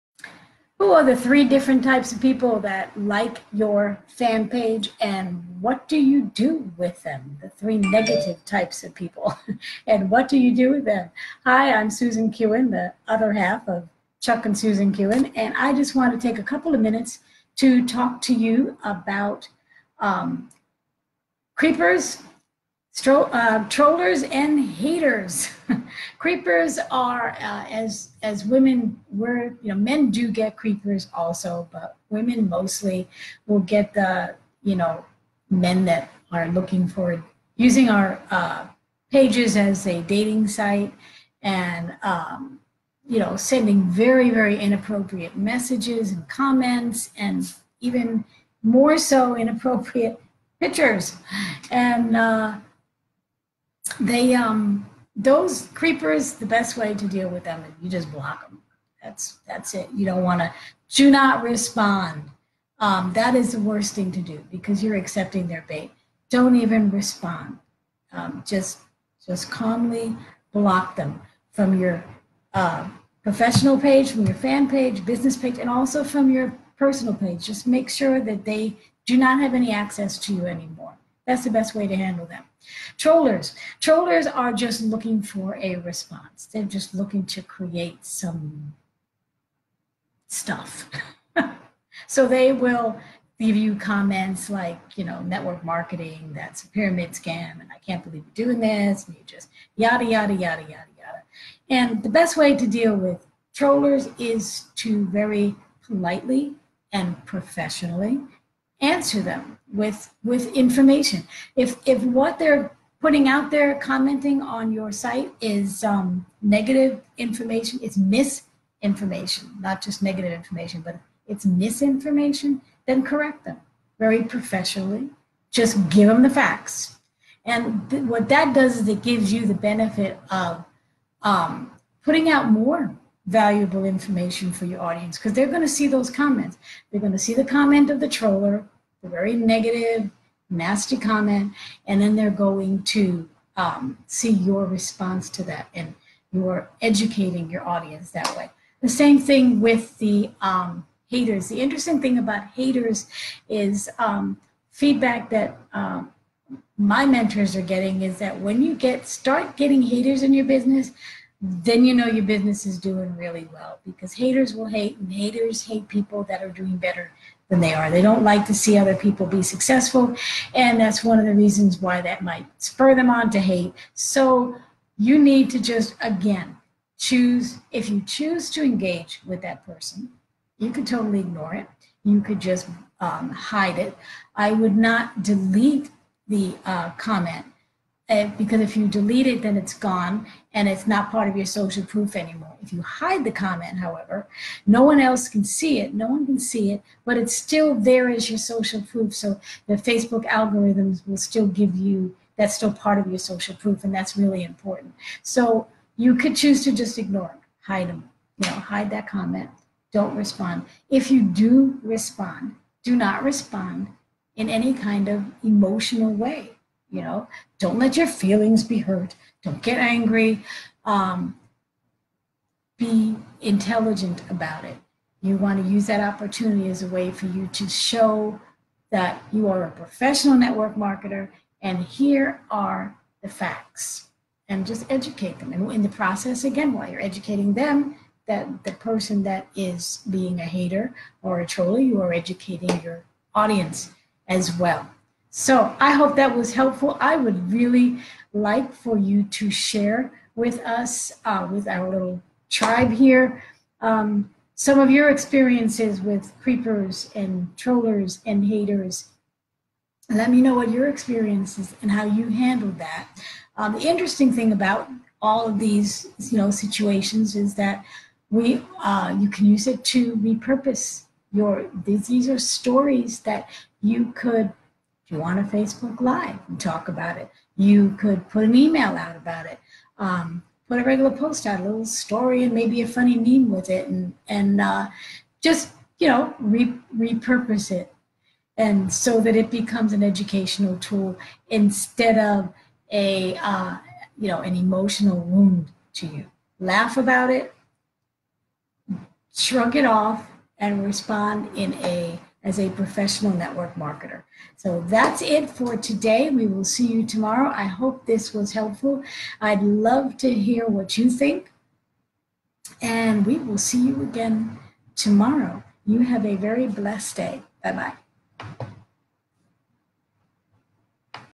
Creepers and Trolls and Haters ...oh my !
3 different people who like your page and how to respond to them.